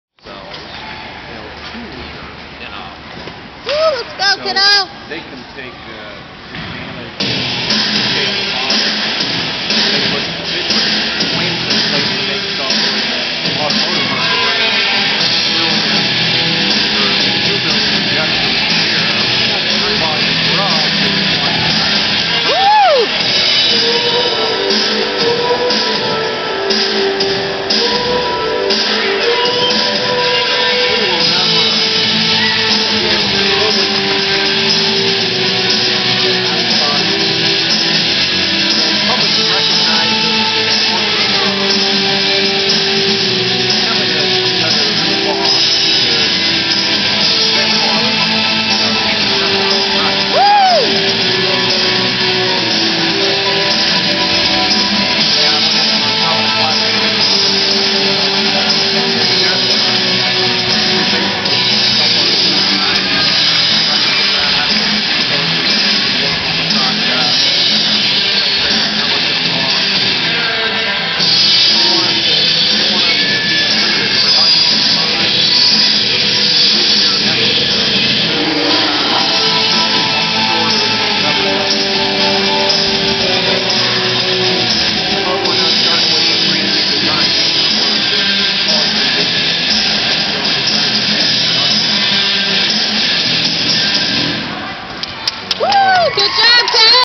Kennedy's Level 9 floor routine
Kennedy Lake from Rising Star Gymnastics, Bloomington IN,  Please view her new routine with more tumbling